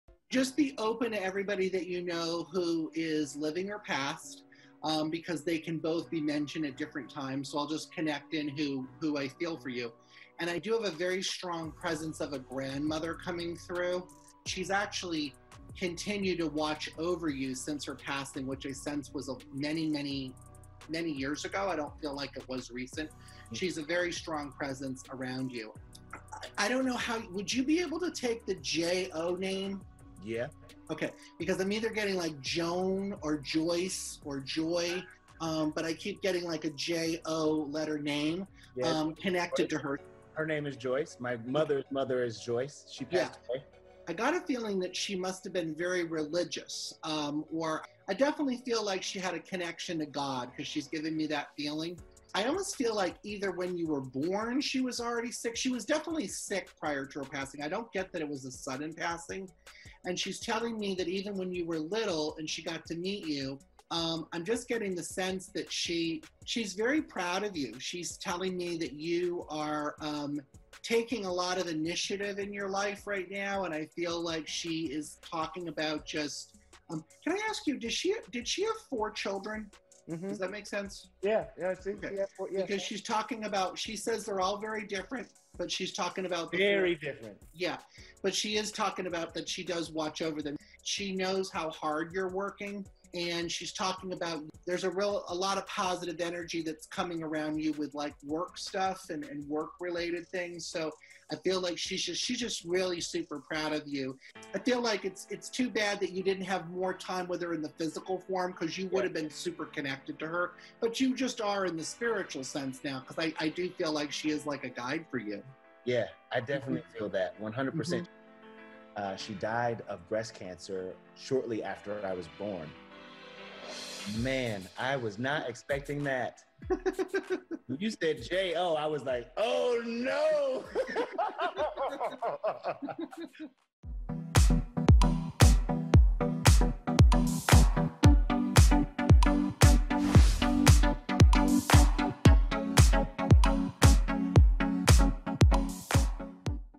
Psychic Thomas John Stuns Scott Evans With Emotional Reading
Thomas John has a new show on CBS All Access called "The Thomas John Experience," where he uses his psychic abilities to help others connect to lost loved ones. While discussing the program, John gave All Access' Scott Evans a stunning reading, which brought him closer to his late grandmother.